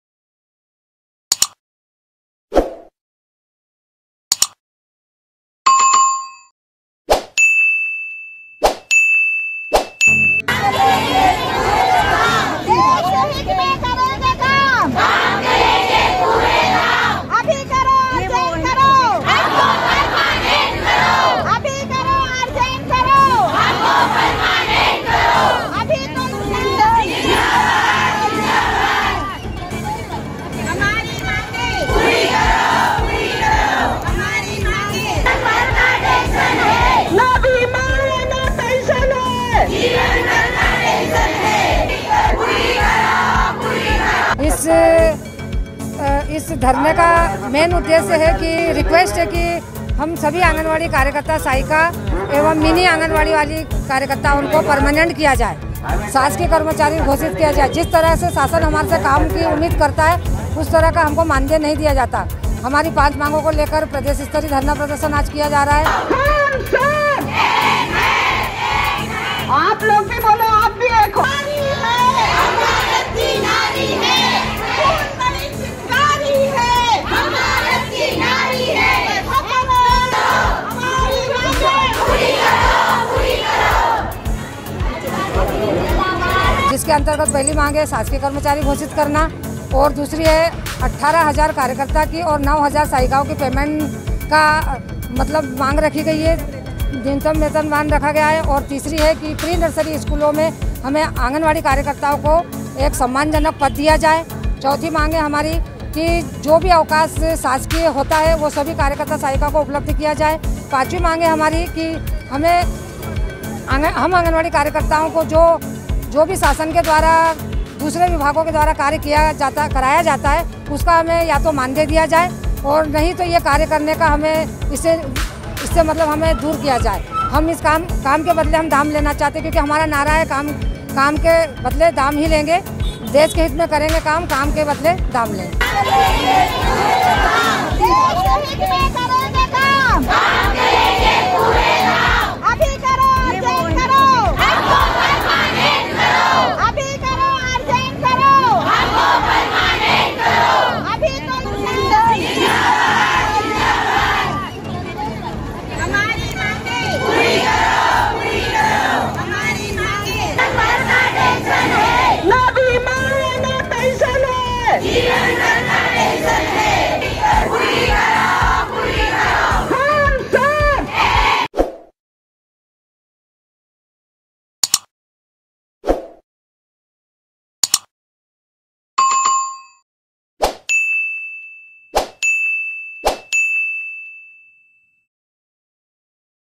शिवराज सरकार के खिलाफ आंगनवाड़ी कार्यकर्ता मैदान में | Anganwadi worker in Madhya Pradesh
खंडवा में आज जिले भर की  आंगनवाड़ी कार्यकर्ता सहायिकाओं ने अपनी मांगों को लेकर धरना दिया। धरने के बाद रैली के रूप में कलेक्टर कार्यालय पहुचे। यहां इन्होंने अपनी सरकारी कर्मचारी घोषित करने मानदेय बढ़ाने जैसी पांच सूत्री मांग को लेकर डिप्टी कलेक्टर को ज्ञापन भी दिया।

आज पूरे जिलेभर की पांच सौ से अधिक आंगनवाड़ी कार्यकर्ता सहायिका ने एसडीएम कार्यलय के पास अपनी मांगों को लेकर धरना दिया । धरने के बाद रैली के रूप में नारेबाजी करते हुए कलेक्टर कार्यालय  पहुचे। यहां इन्होंने अपनी पांच सूत्री मांगों को लेकर डिप्टी कलेक्टर अनुभा जैन को ज्ञापन दिया । आंगनवाड़ी कार्यकर्ता सहायिकाओं ने ज्ञापन के माध्यम से सरकारी कर्मचारी घोषित करने कार्यकर्ताओ का मानदेय 18 हजार रुपये और सहायिकाओं का 9 हजार रुपये किए जाने के साथ अन्य मांग शासन से की है ।